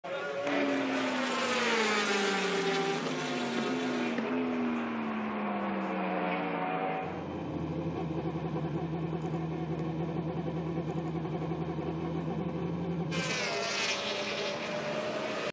Jonathan Rea's reaction to Race 1 victory at Phillip Island 🔥
POV: You take the victory after a 2️⃣4️⃣ races win-drought 🥇

Bravo, Jonathan Rea 👏